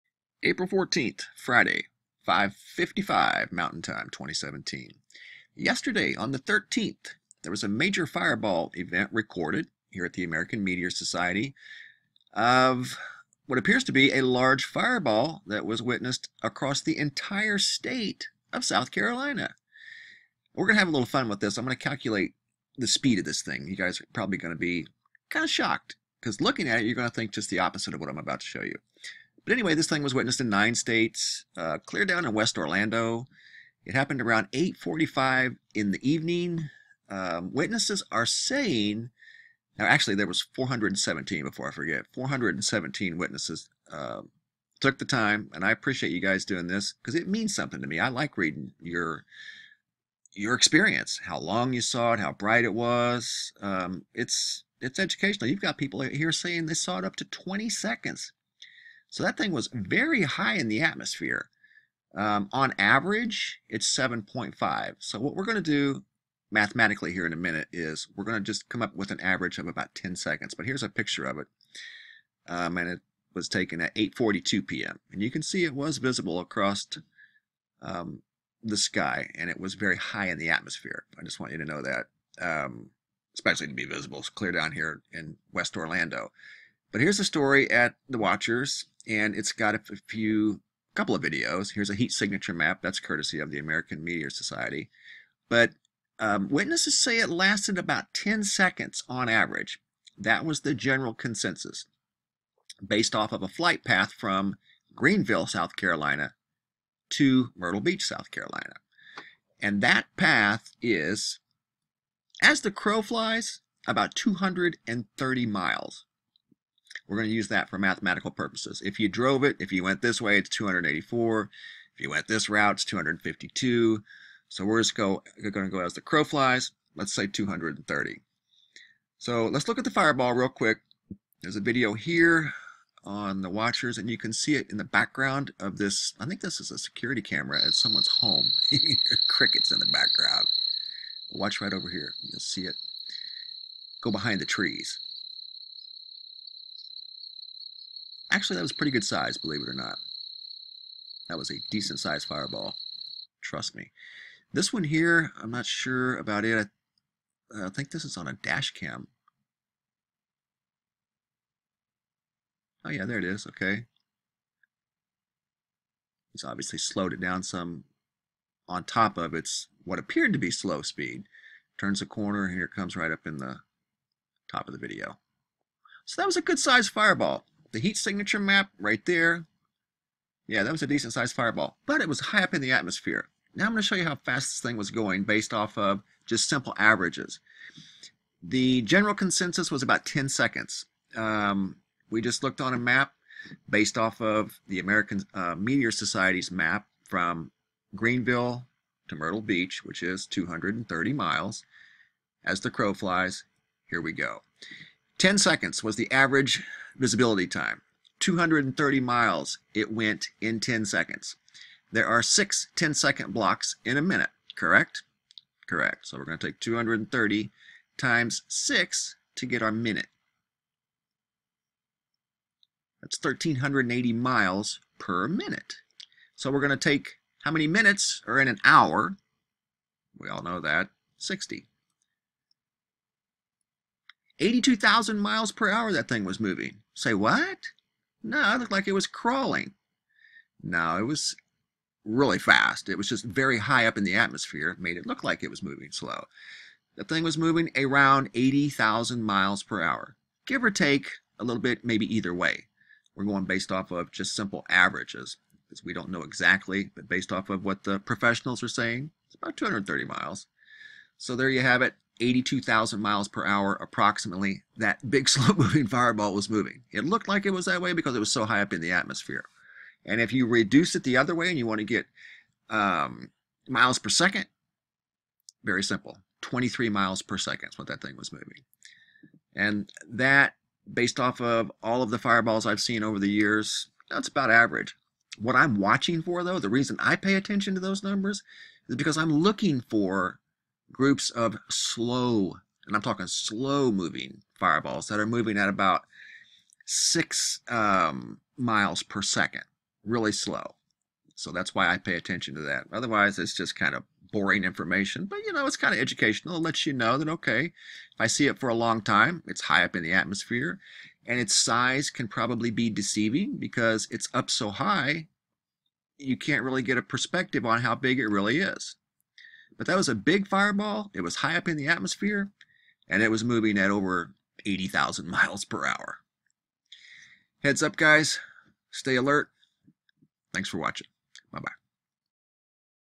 82,000 mph Fireball Blazes through S.C. Sky | A true "High Roller"
April 14, 2017: A high altitude, what 'appeared' to be a slow moving fireball was witnessed by probably thousands of people, reported by 417 people from 9 different states at the American Meteor Society. How fast was it moving? We calculate in the video based off of the overall average given by the fine folks that took the time to share their experience with the AMS. (report any meteors you see at their website, its very user friendly and your input is important to understanding the event) link below... (bookmark it!)
(submit your fireball sightings here)